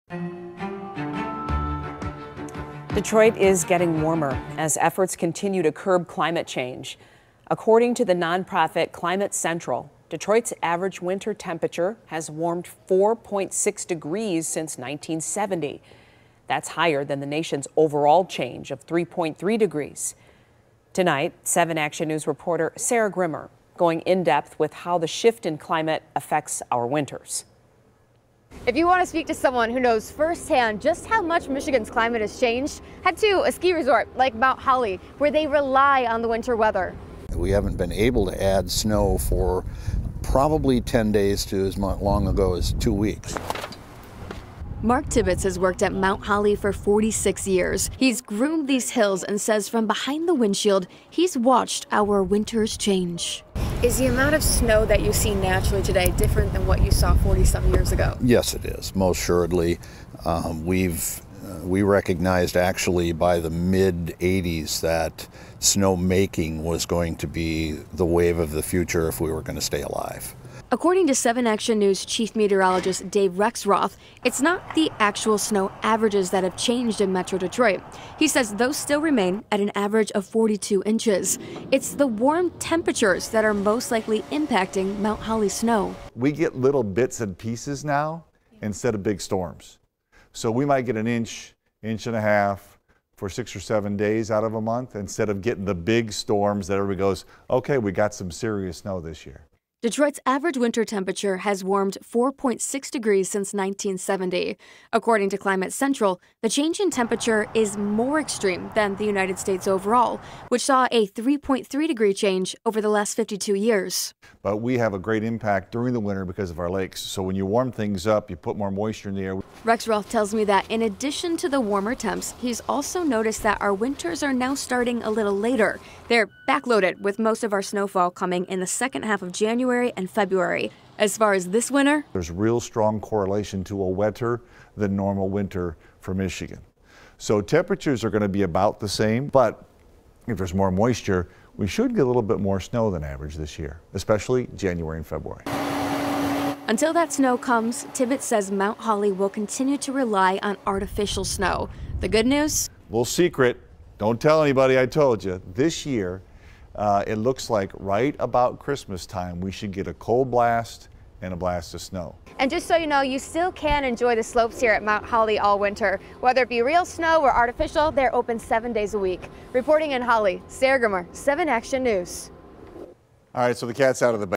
Average Detroit temperature soars almost 5 degrees in 50 years. What that means for our winters
Detroit's average winter temperature has warmed 4.6 degrees since 1970, according to Climate Central.